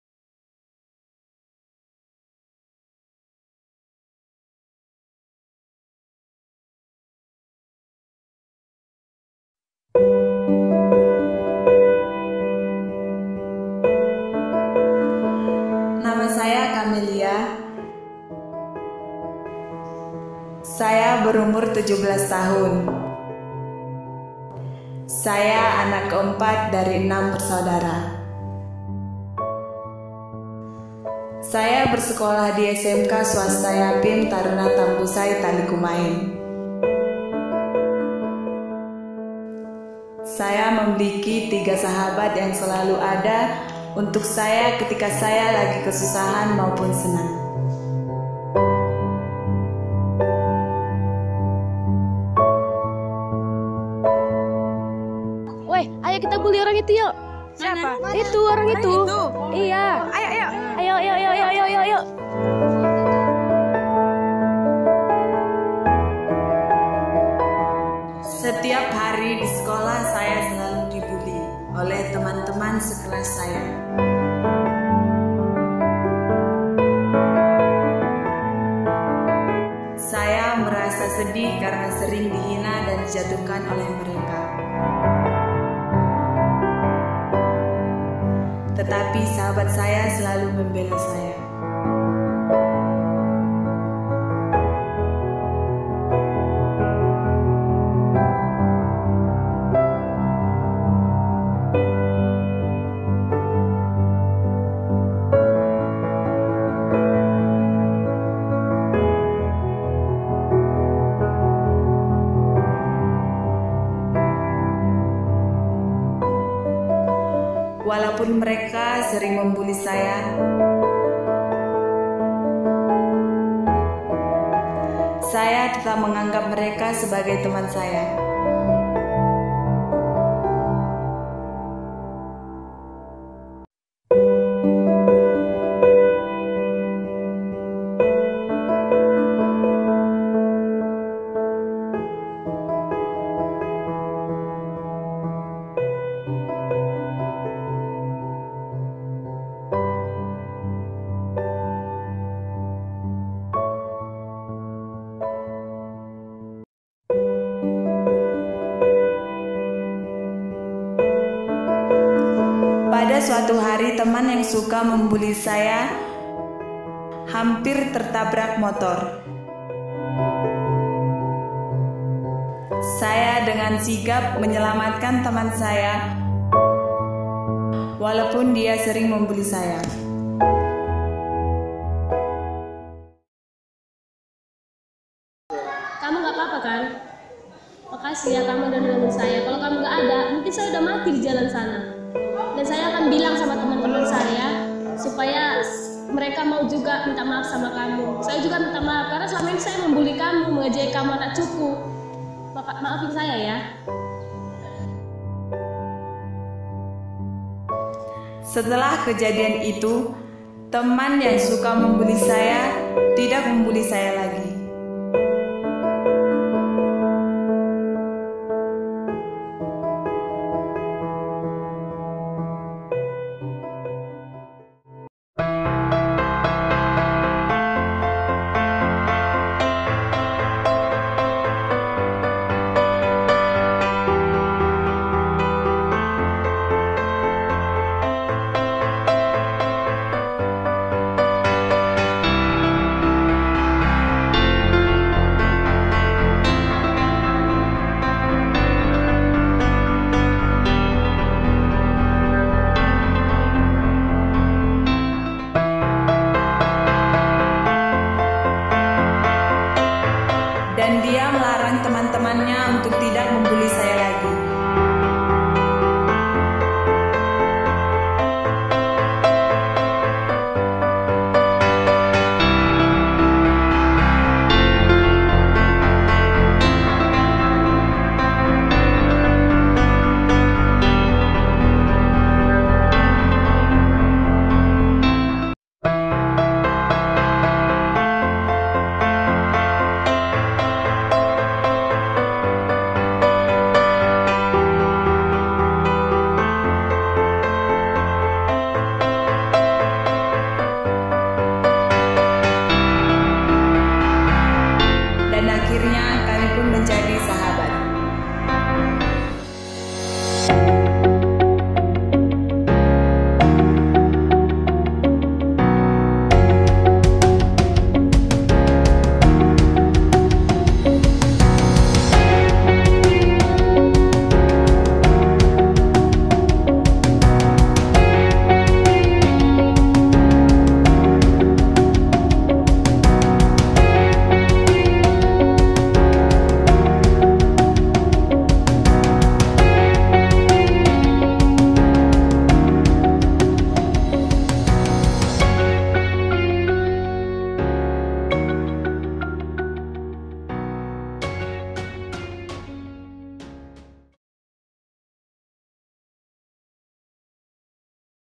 New MOVIE *Smk S Yapim Taruna Tambusai* =Persahabatan= Kelas
cerita ini hanya fiki belaka mohon maaf  apabila ada kesamaan cerita, ini hanya ajang dalam mengembangkan bakat siswa dalam dunia perfileman, serta kreatifitas mereka setelah mereka tamat...